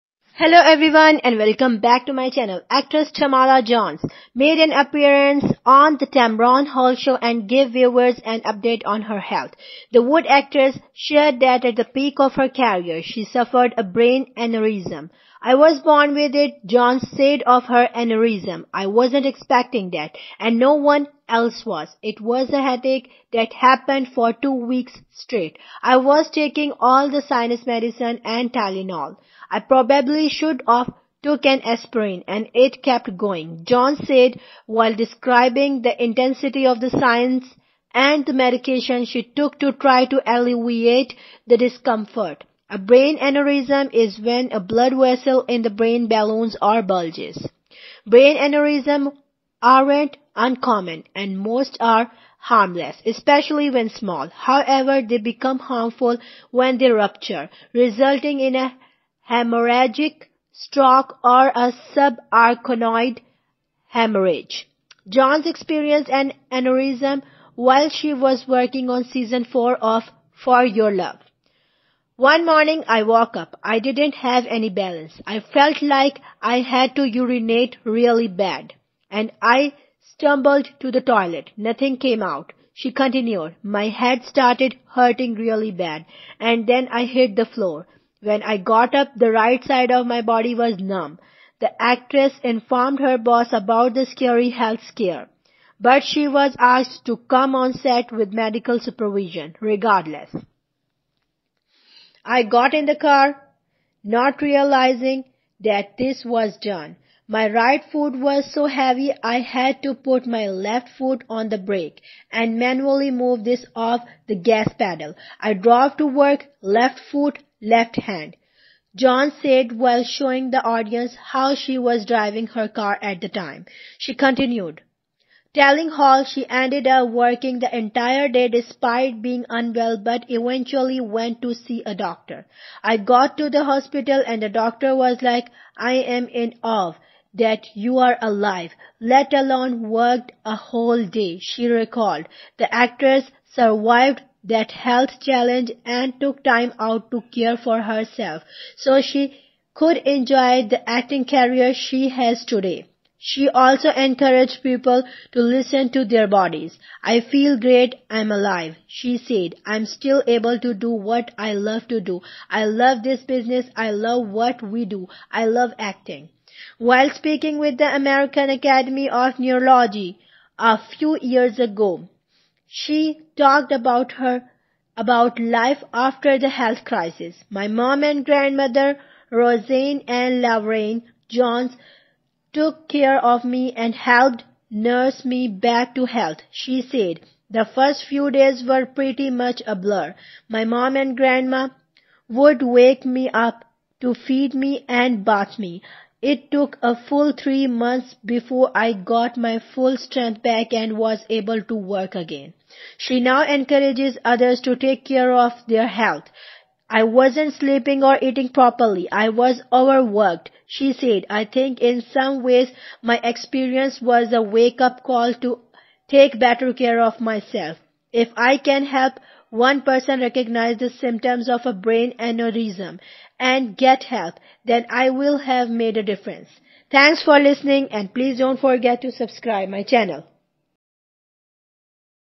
Tamala Jones Opens Up About Her Scary Brain Aneurysm.
Tamala Reneé Jones is an American actress. She is known for her roles in films such as Booty Call, The Wood, Kingdom Come, The Brothers, and What Men Want.